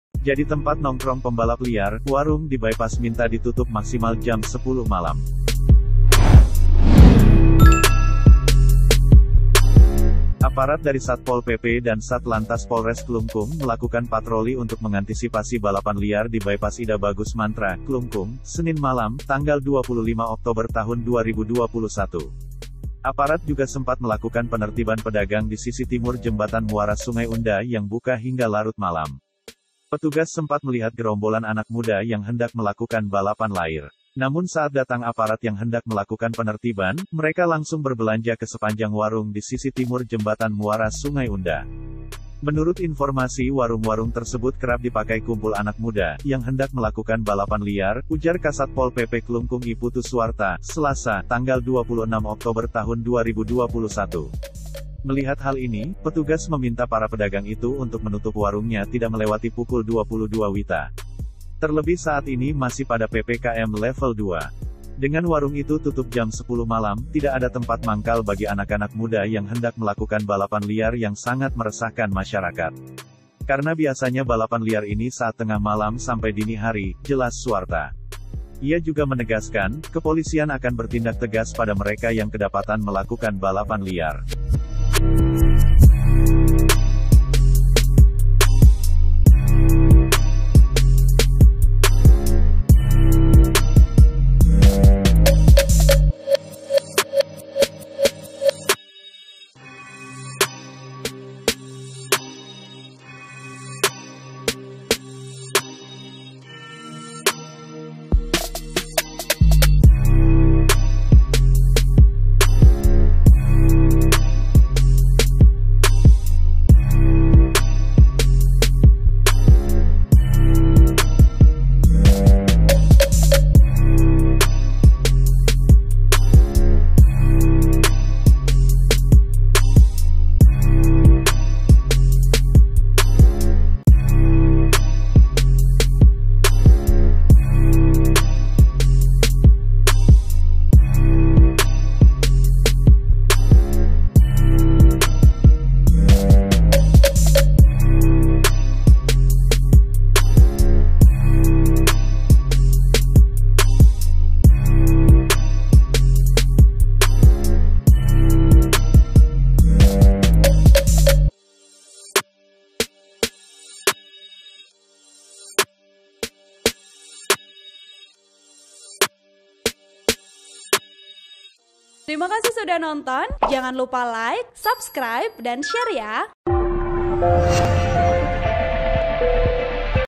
Jadi Tempat Nongkrong Pembalap Liar, Warung di Bypass Minta Ditutup Maksimal Jam 10 Malam
Aparat dari Satpol PP dan Sat Lantas Polres Klungkung melakukan patroli untuk mengantisipasi balapan liar di Bypass Ida Bagus Mantra, Klungkung, Senin malam (25 Oktober 2021).
Aparat juga sempat melakukan penertiban pedagang di sisi timur jembatan muara Sungai Unda yang buka hingga larut malam.

(IAG)